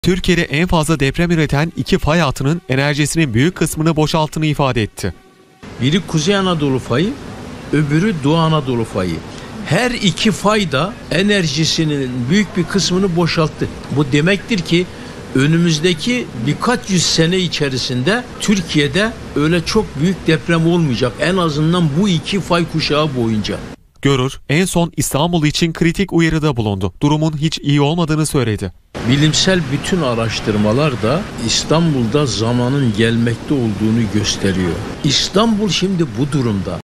Prof. Dr. Naci Görür'den rahatlatan açıklama
Naci Görür: İki fay hattı enerjisini boşalttı